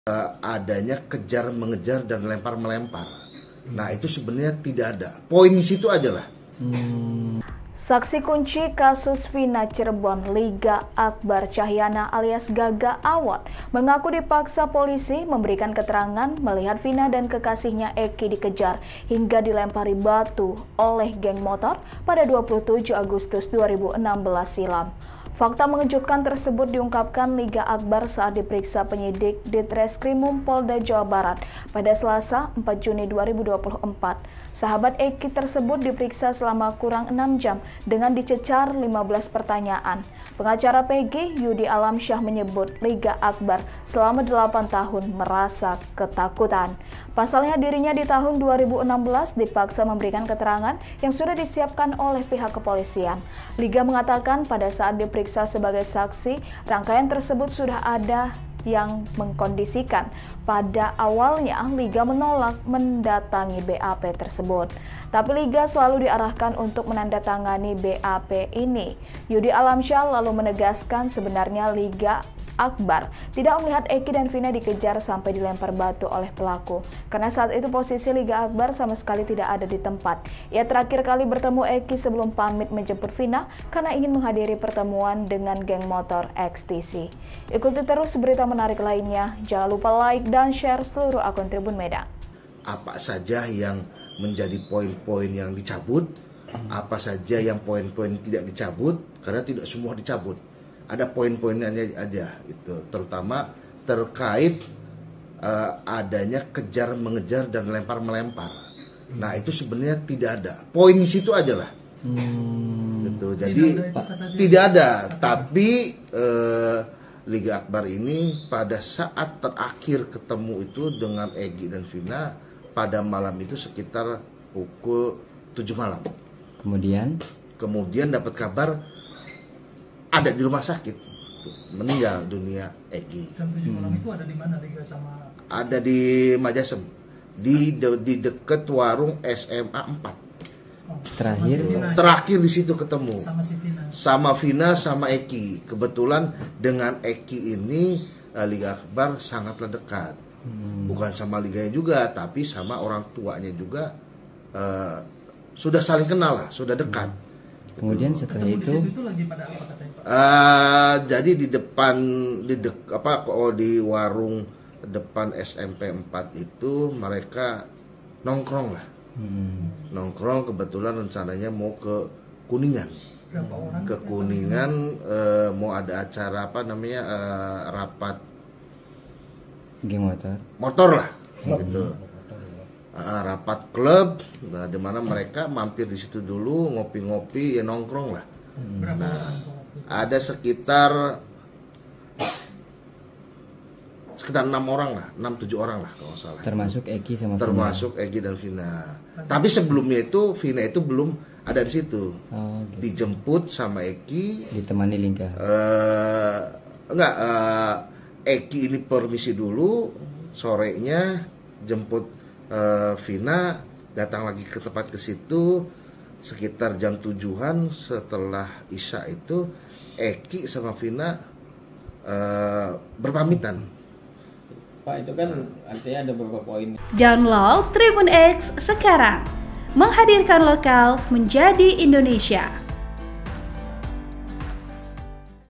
BERANI! SAKSI NGAKU Dipaksa Polisi Beri Keterangan Lihat Vina dan Eky Dilempari Batu, Kini Cabut BAP
TRIBUN-MEDAN.COM - Saksi kunci kasus Vina Cirebon, Liga Akbar Cahyana alias Gaga Awod mengaku dipaksa Polisi memberikan keterangan melihat Vina dan kekasihnya Eky dikejar hingga dilempari batu oleh geng motor, pada 27 Agustus 2016 silam.

Fakta mengejutkan tersebut diungkapkan Liga Akbar saat diperiksa penyidik Ditreskrimum Polda Jawa Barat pada Selasa (4/6/2024).

Sahabat Eky tersebut diperiksa selama kurang 6 jam dengan dicecar 15 pertanyaan.

Host : Joy Silvana Aritonang
Editor Video : Gita NPT